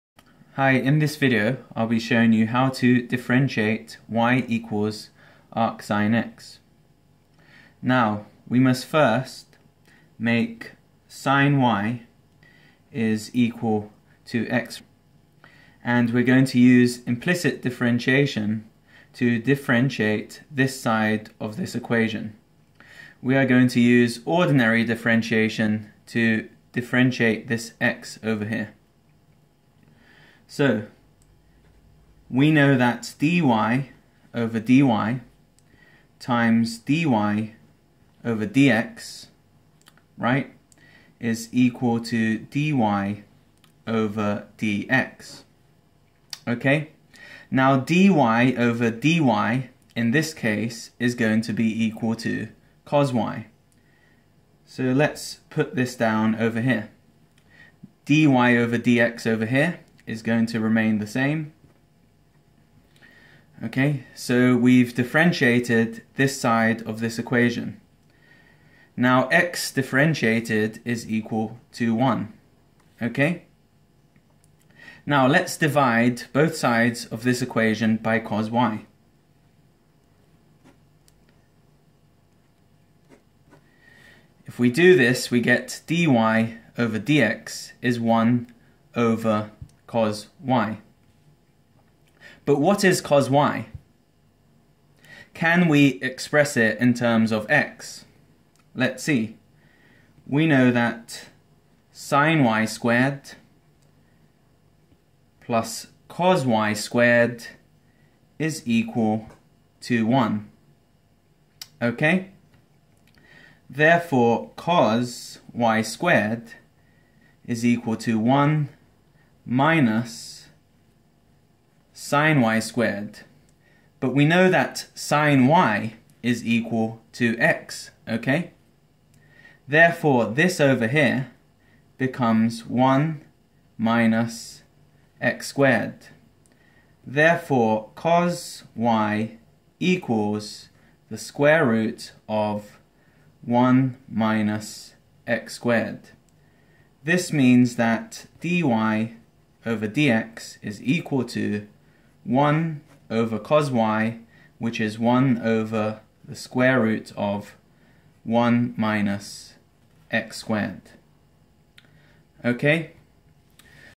How to differentiate y=arcsinx (Implicit Differentiation)
y=arcsinx

siny=x

cosy * dy/dx = 1

dy/dx = 1/cosy

But...

(siny)^2+(cosy)^2=1

(cosy)^2=1-(siny)^2

(cosy)^2=1-x^2

cosy=√(1-x^2)

Therefore:

dy/dx = 1/(√(1-x^2))